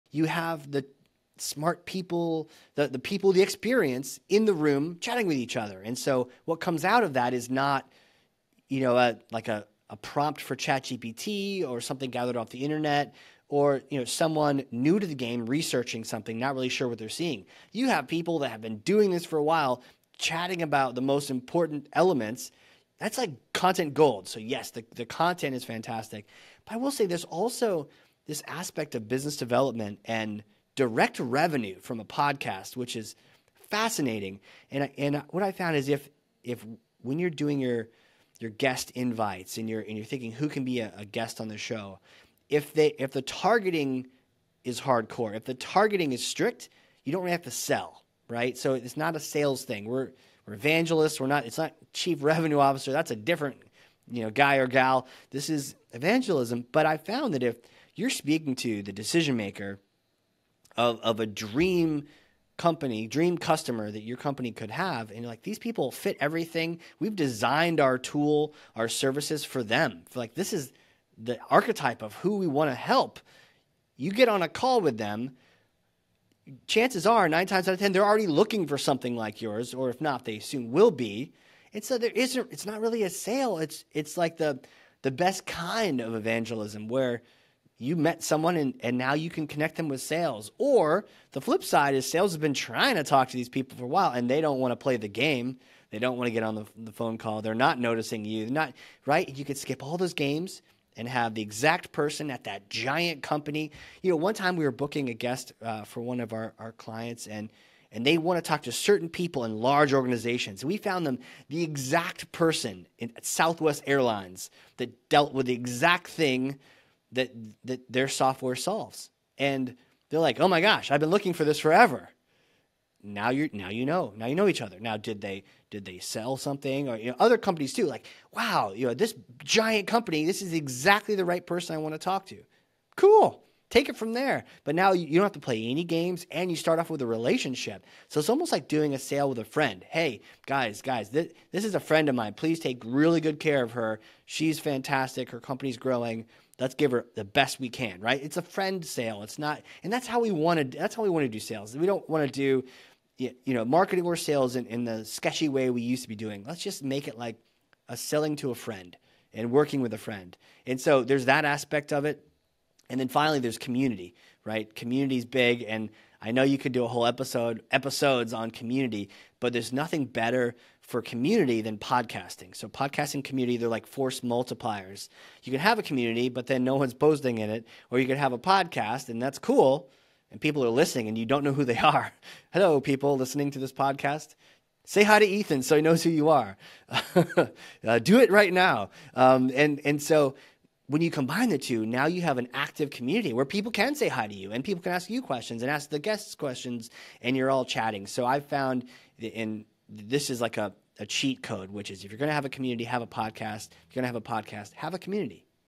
Podcasting for Content, Revenue, and Community with Casey Cheshire (Ringmaster) - Ep 035 Highlight 7
In this clip: Casey breaks down three specific benefits of starting a podcast - content creation, revenue generation, and community engagement.

In this episode: Casey Cheshire, Founder and Chief Evangelist at Ringmaster, joins host Ethan Beute to share why every company needs an evangelist - and a podcast!

Casey explains how companies can leverage podcasts to connect with their ideal customers, as well as the benefits of podcasting in terms of capturing, learning from, and sharing the literal voice of the customer.

Chief Evangelist is produced by Ringmaster, on a mission to create connections through B2B podcasts.